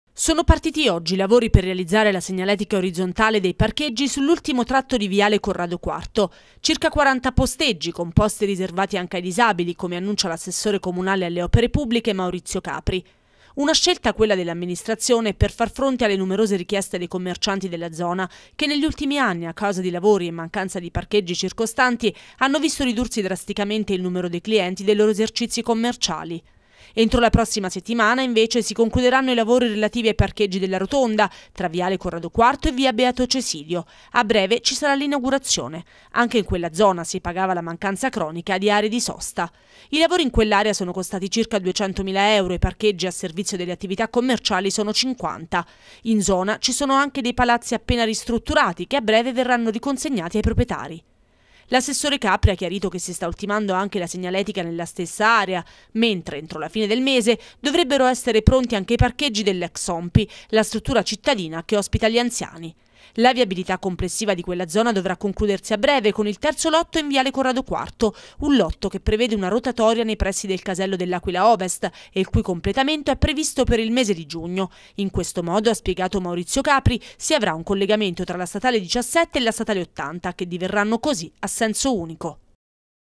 Viabilità: piccola rivoluzione a L'Aquila
Nuovi parcheggi e modifiche alla viabilità all'Aquila. I dettagli nel servizio di Daniela Rosone